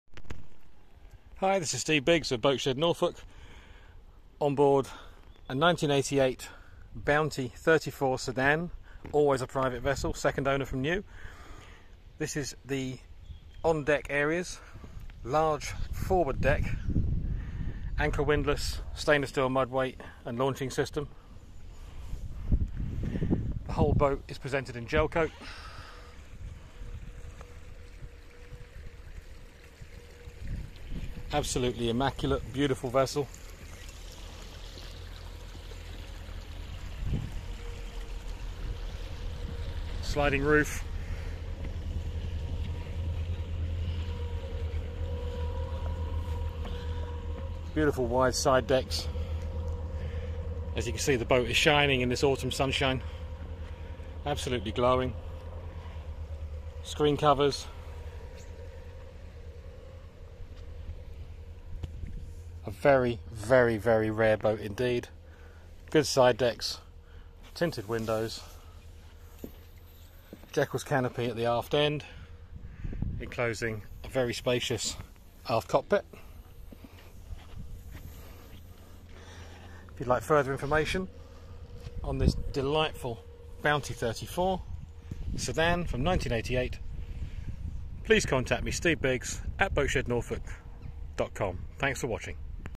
Bounty 34 sedan twin screw - Boatshed - Boat Ref#315272
Bounty 34 sedan twin screw for sale with Boatshed Norfolk Photos and video taken by Boatshed Norfolk